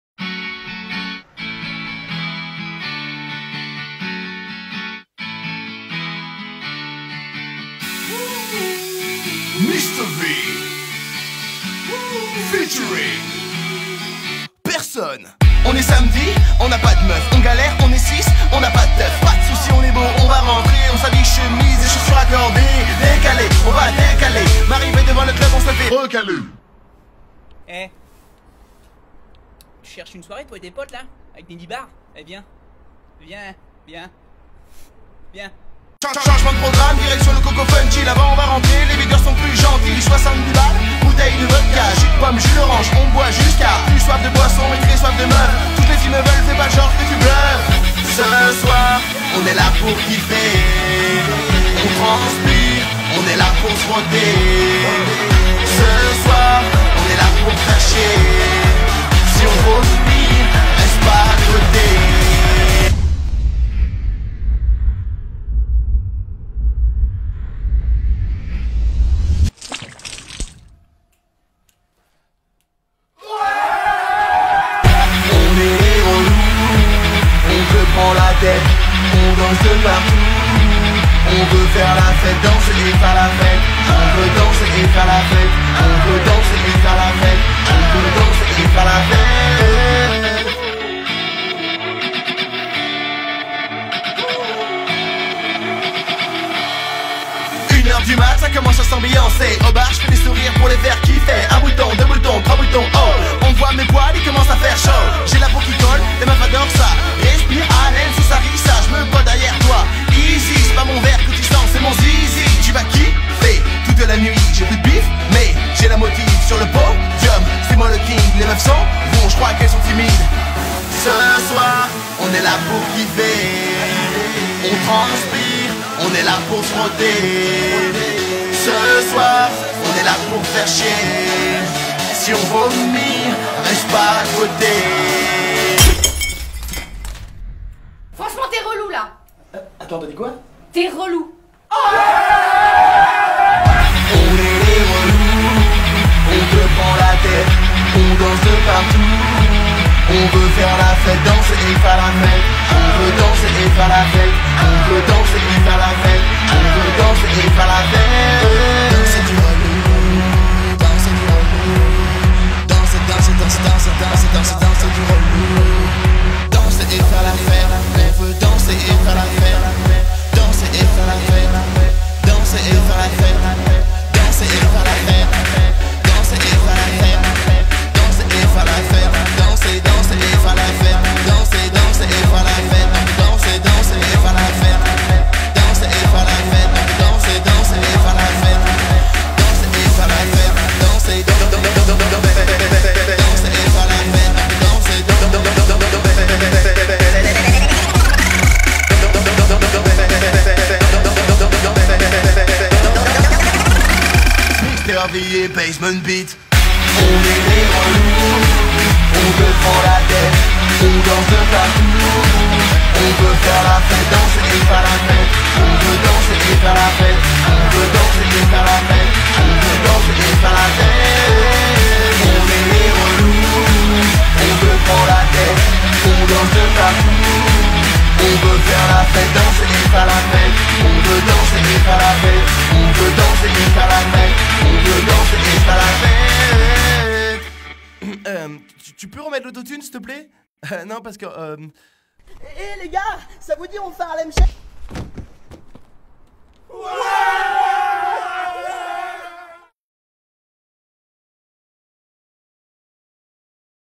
Romain JB
Mister V qui chante au coco funky